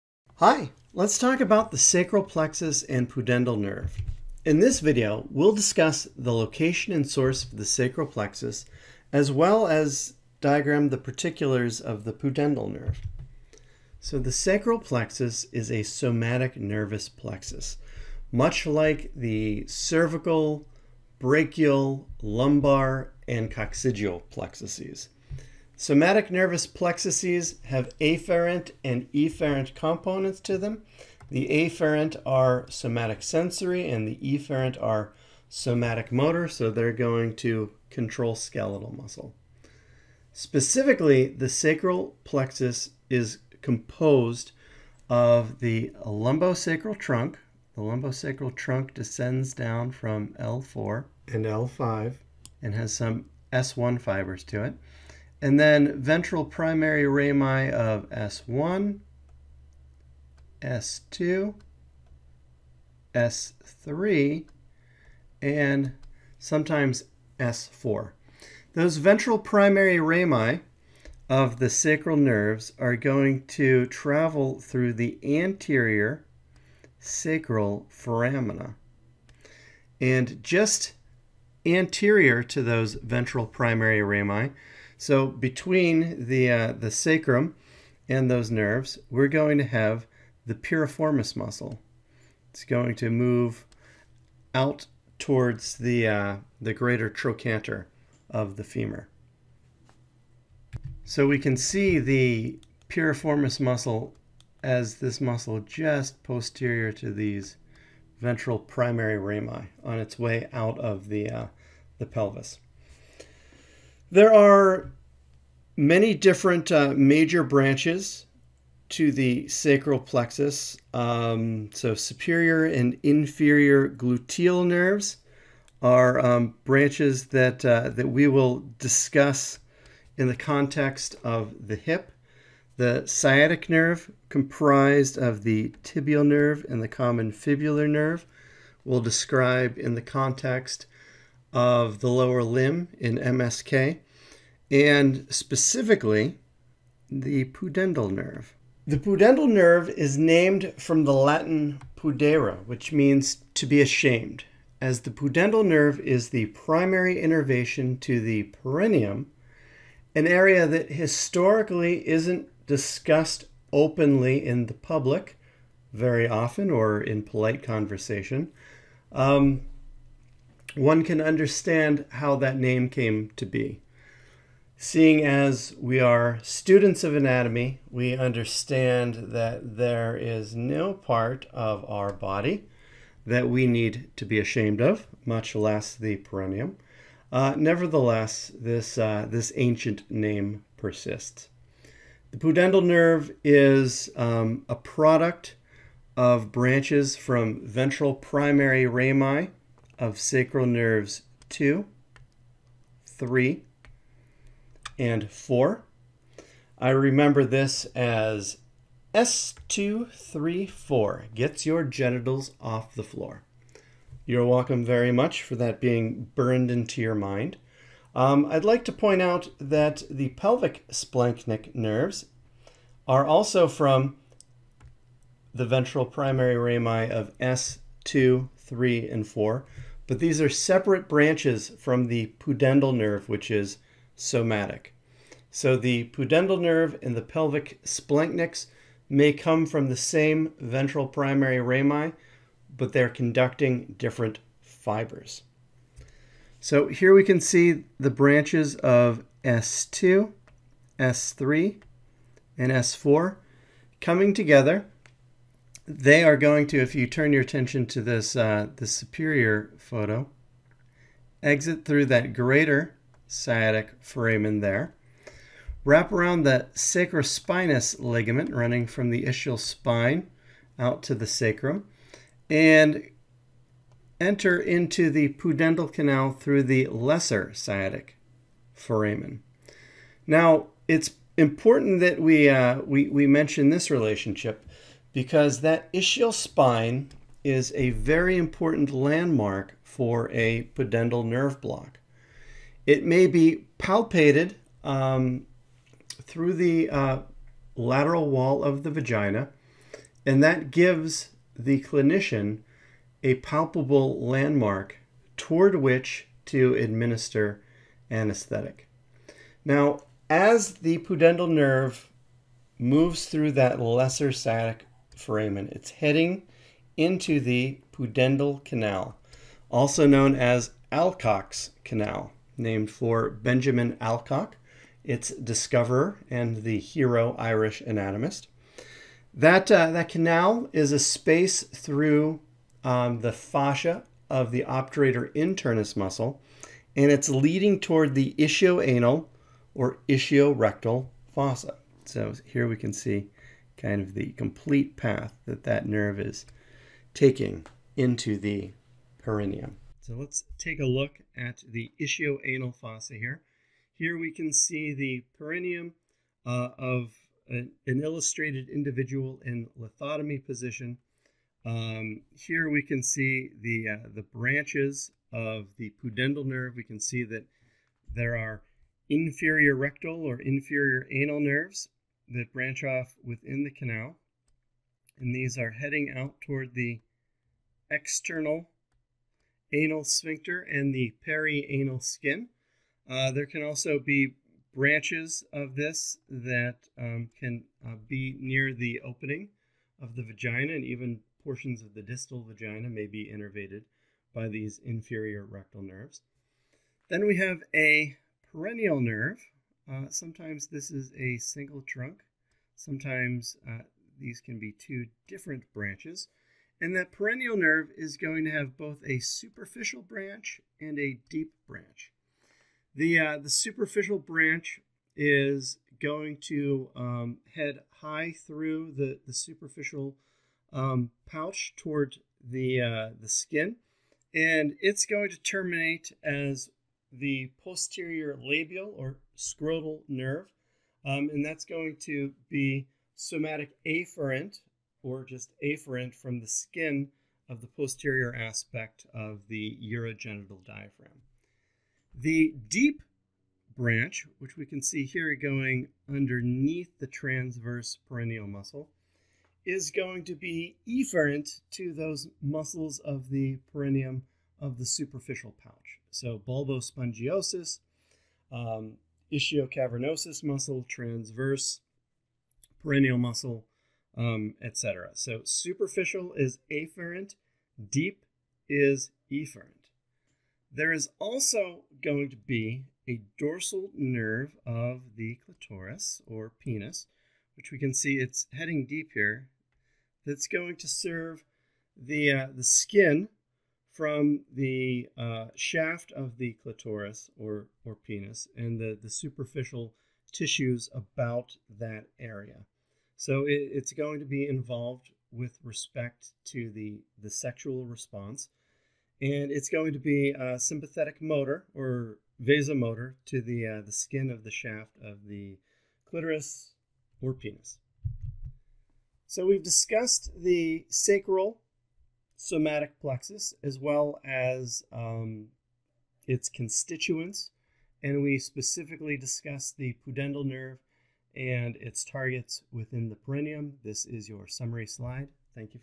Sacral plexus - M1 Perineum
Dr. Glenn Fox discusses the sacral plexus as part of the Learning Objective video series for the First Year Medical Gross Anatomy course at the University of Michigan.